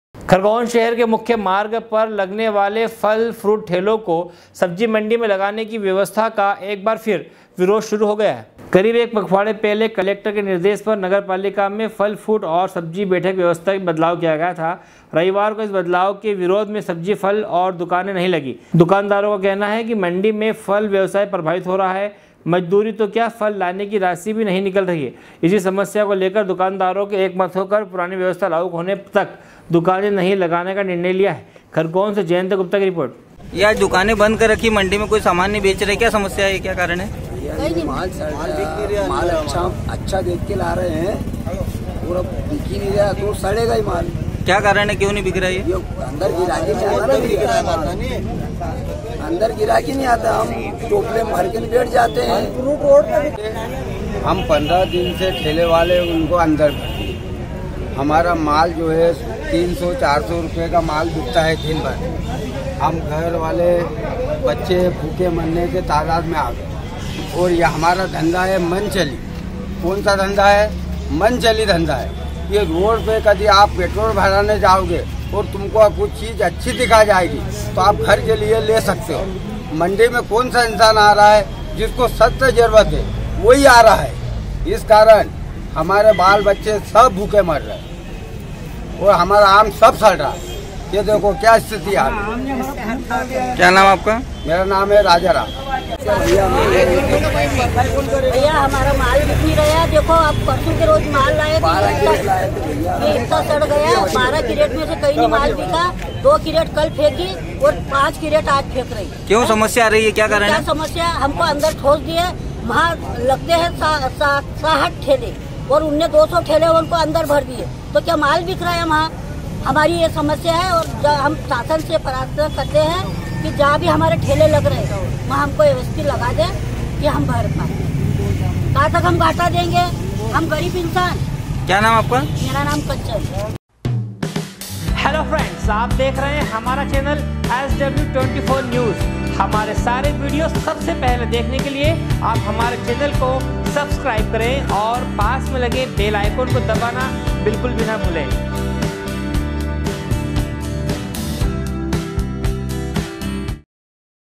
खरगोन के मुख्य मार्गाे पर लगने वाले ठेलो को सब्जी मंडी में लगाने का एक बार फिर वीरोध शुरू हो गया है।
खरगोन शहर के मुख्य मार्गाे पर लगने वाले फल फ्रूट ठेलो को सब्जी मंडी में लगाने की व्यवस्था का एक बार फिर वीरोध शुरू हो गया है। खरगोन से जयन्त गुप्ता की रिर्पोट।